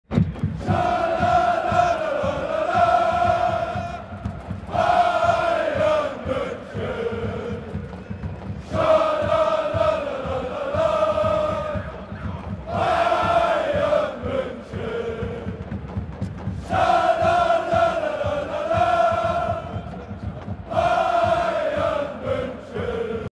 Südkurve Bayern Munich vs AS Roma
During the game in the Südkurve between all the crazy Bayern Fans!